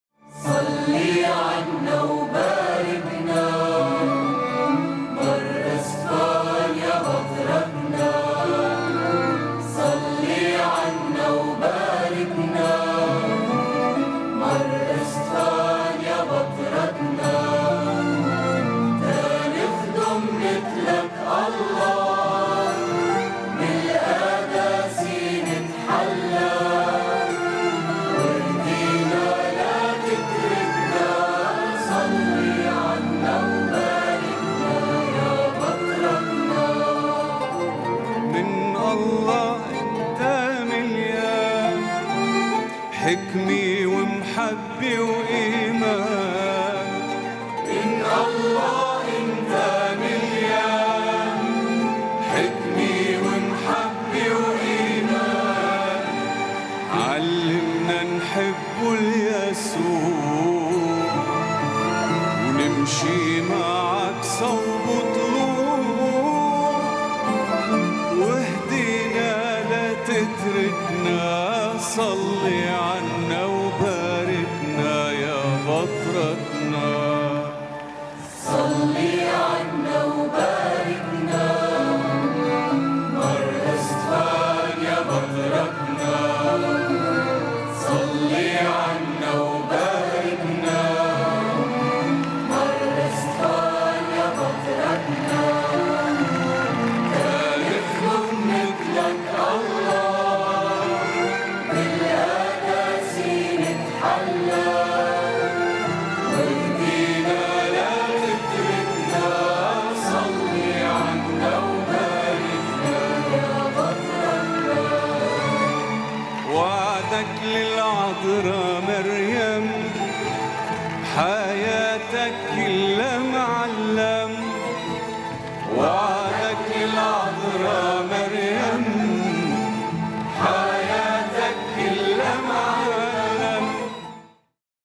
دخول ذخائر الطوباوي الجديد البطريرك مار اسطفان الدويهي إلى الصرح البطريركي في بكركي
فليُباركنا الرب بشفاعته!